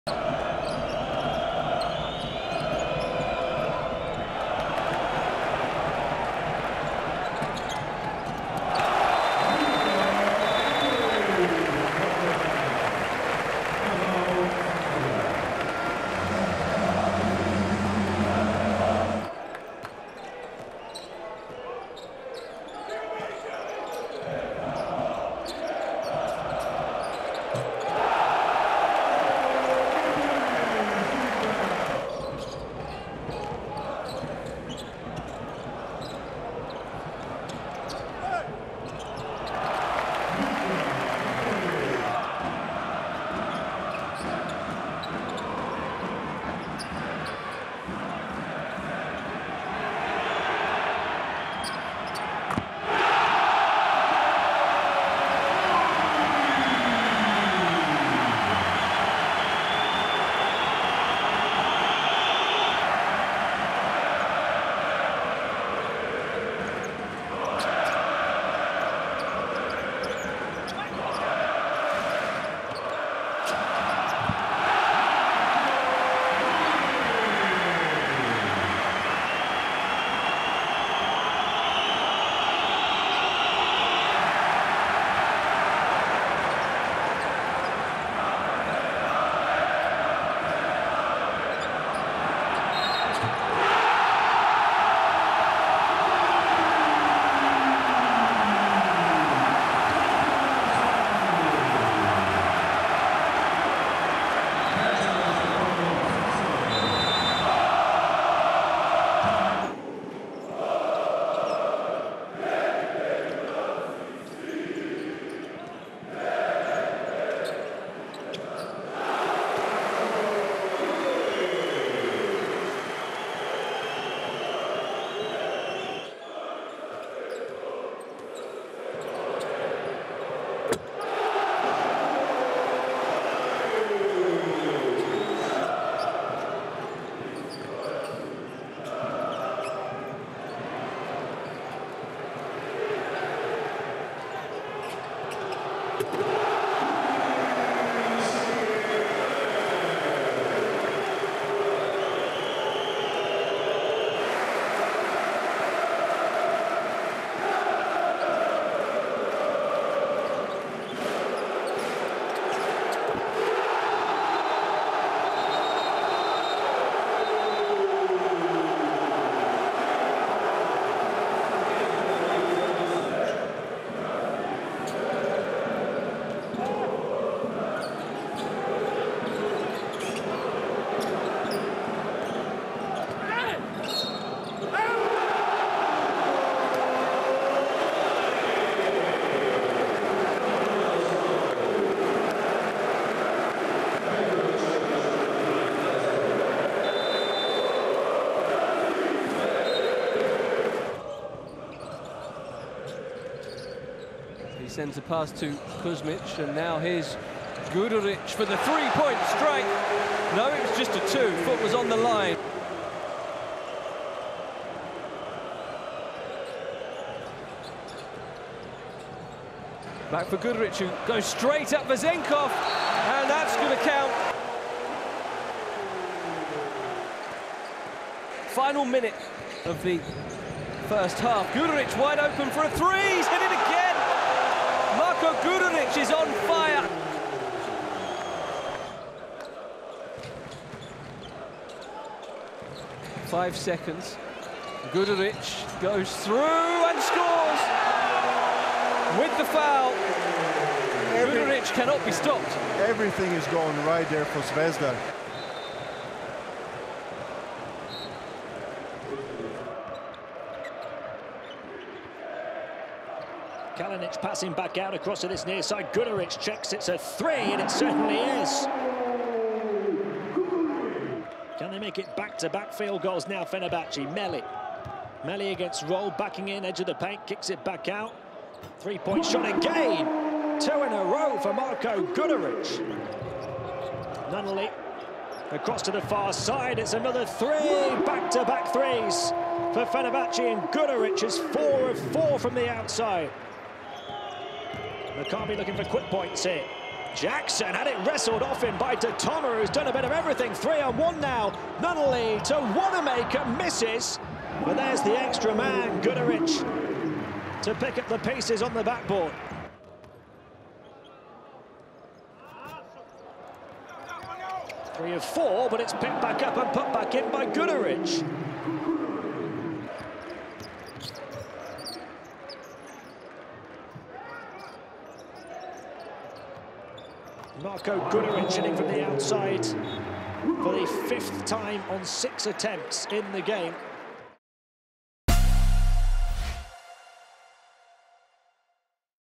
Lookback: Guduric revisits Zvezda
After he rejoined his last Turkish Airlines EuroLeague team, Fenerbahce Beko Istanbul, at the end of 2020, shooting guard Marko Guduric will mark the start of 2021 by traveling home to Serbia on Thursday to play against the team with whom he debuted in the competition, Crvena Zvezda mts Belgrade.